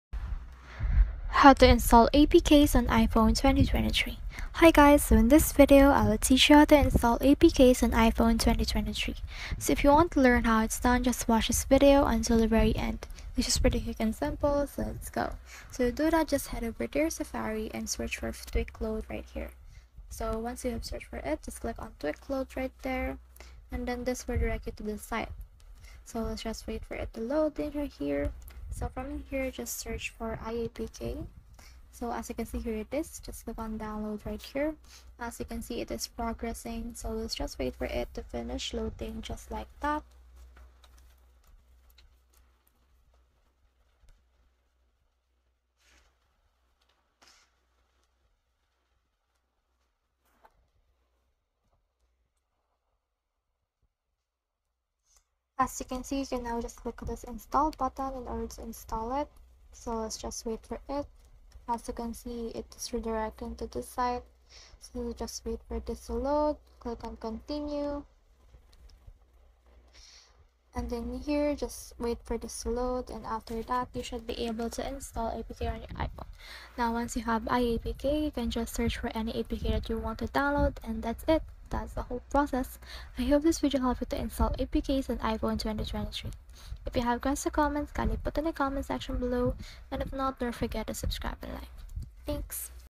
How To Install APKs On iPhone 2023 SIMPLE
Looking to install APKs on your iPhone in 2023? While iPhones primarily use the App Store for app installations, we'll show you a straightforward method to sideload APKs without the need for jailbreaking.

In this quick guide, we'll provide simple steps to install APKs on your iPhone using a trusted tool. Whether you want to access apps unavailable on the App Store or try out unique software, our instructions will help you get started.

No need for complicated procedures or technical expertise—watch this video and discover a simple and effective way to install APKs on your iPhone in 2023. Explore new possibilities on your iOS device with ease!